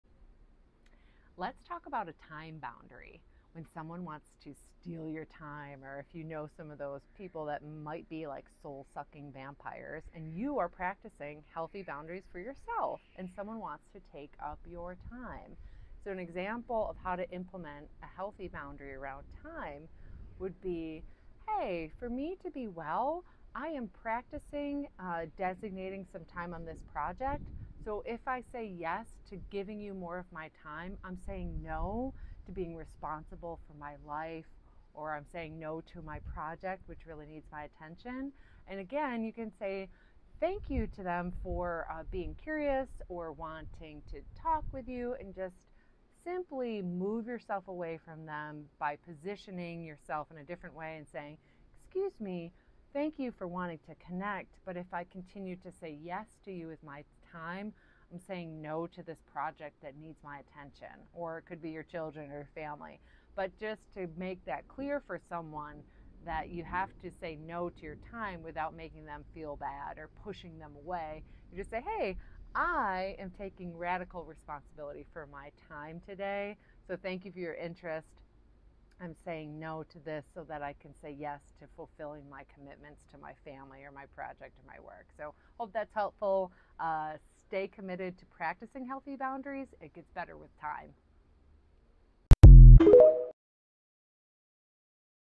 Time Boundary
Let's talk about a time boundary!! How you utilize time boundaries throughout your day? 🕰️